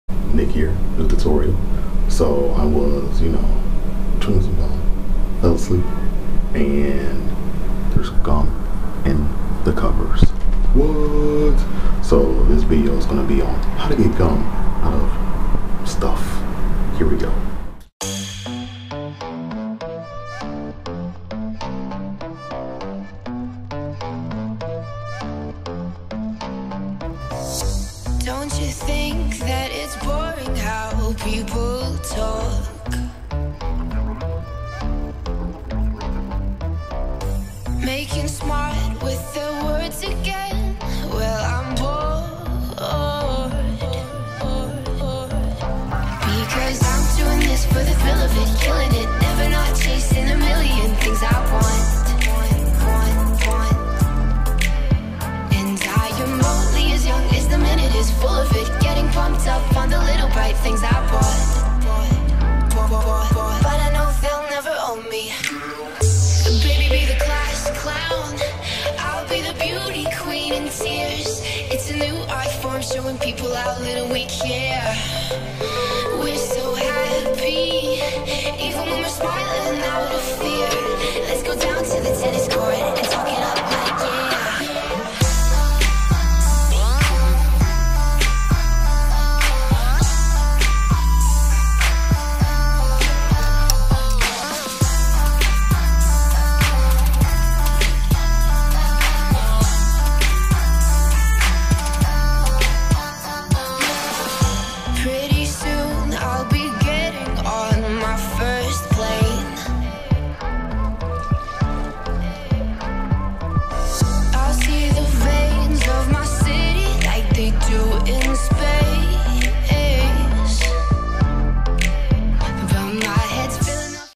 Remove gum : Quick tutorial
How to remove gum from fabric. In my situation bed sheet.

What you need:
Hot water
Shampoo (clear color, prevent staining)